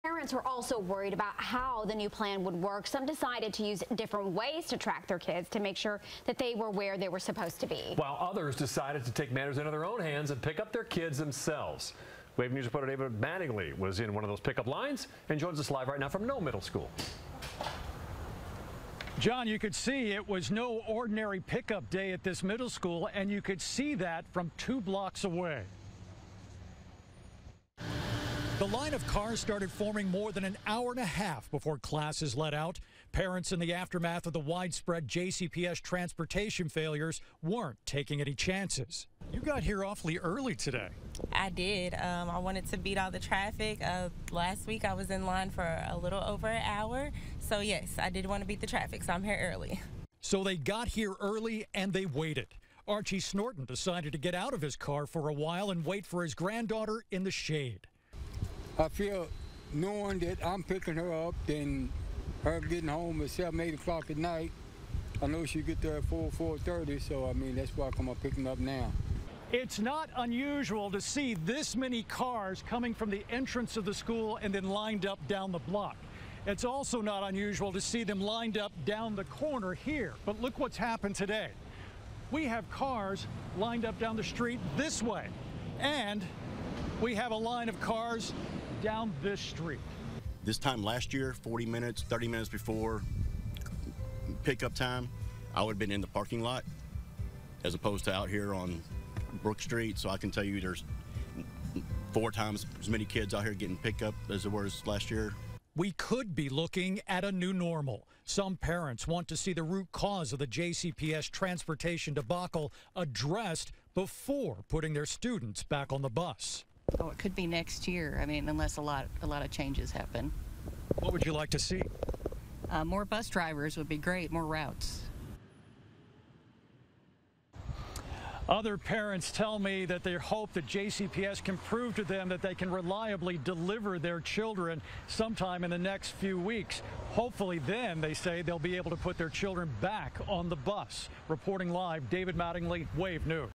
Extra long lines at pickup lanes as JCPS parents avoid putting students on buses
The line of cars started forming at Noe Middle School more than an hour and a half before classes let out.